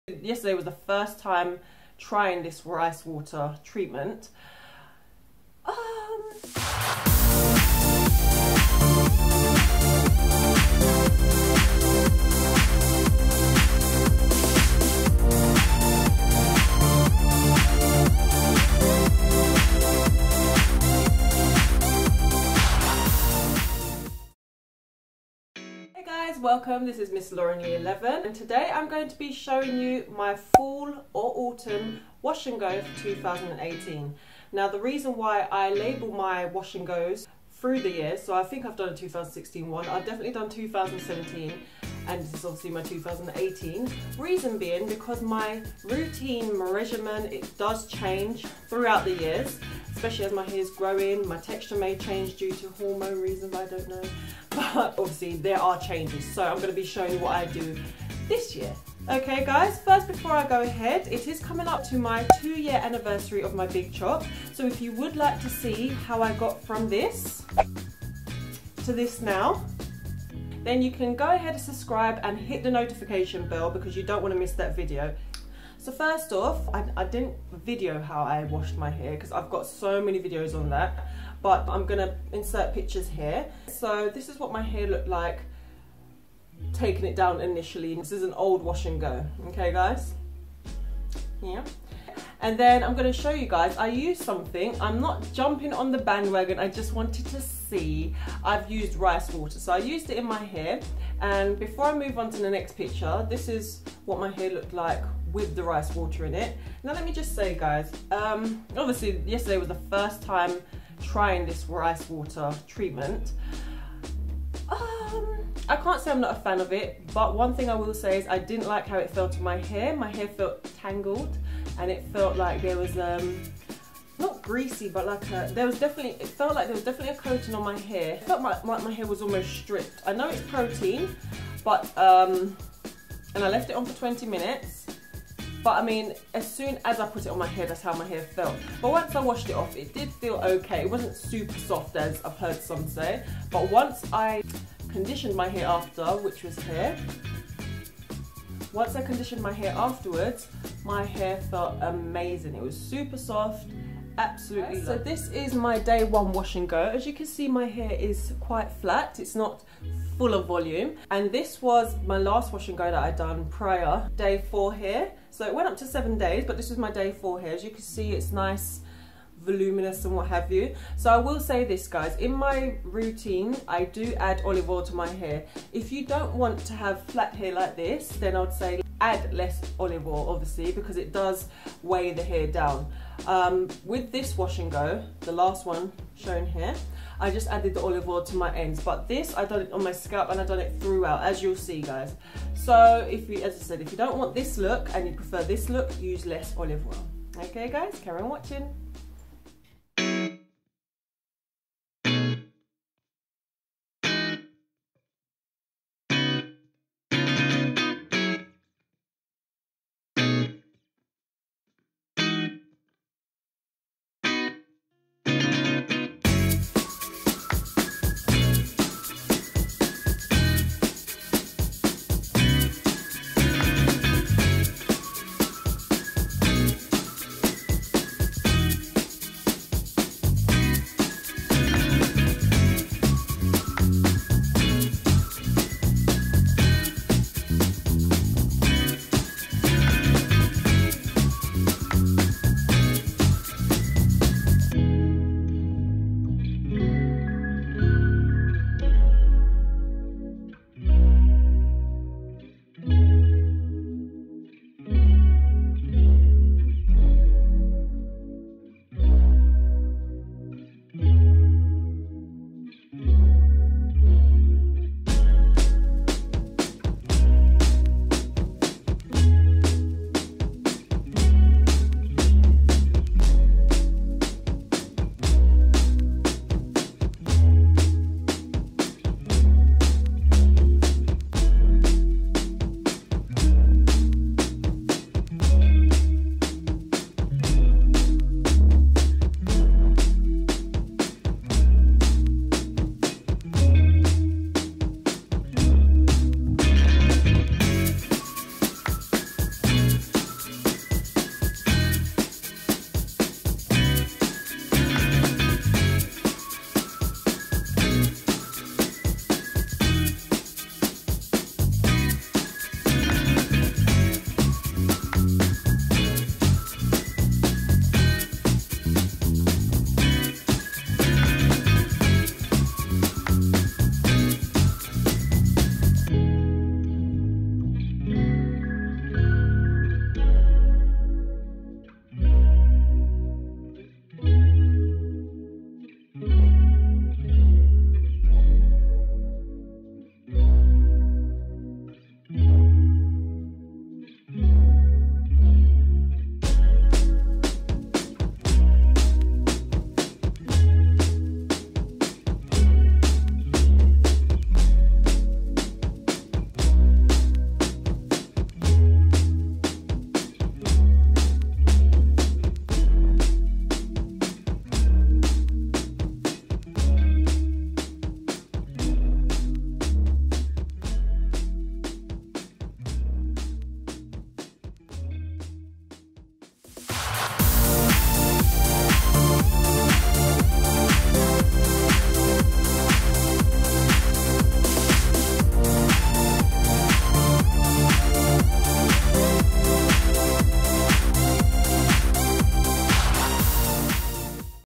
Wash N Go Natural Hair | FALL ROUTINE 2018
It's that time again to show you my new Wash N Go routine for fall/autumn 2018.
My wash n go routines, always change, due to various factors, however, I always try to maintain moisture and definition throughout.